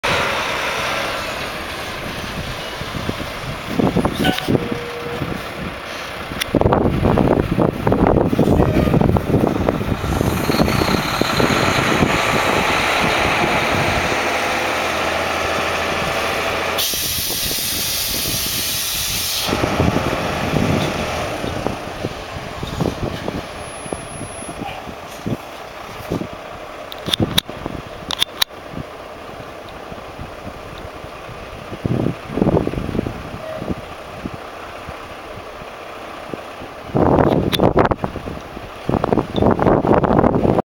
Nitrogen Stinger on Fontaine Lowboy Trailer - TM Trailer Sales, Inc. - YOUR LOWBOY TRAILER DEALER
TM Trailer Sales, Inc. - YOUR LOWBOY TRAILER DEALER - lowboydealer.com

Demonstrating a Nitrogen Charged Stinger manufactured by Superior Trailer on a Fontaine Lowboy Trailer.

Nitrogen Charged Stinger will adapt to all makes of trailers as well as tub grinders, cranes, and ect.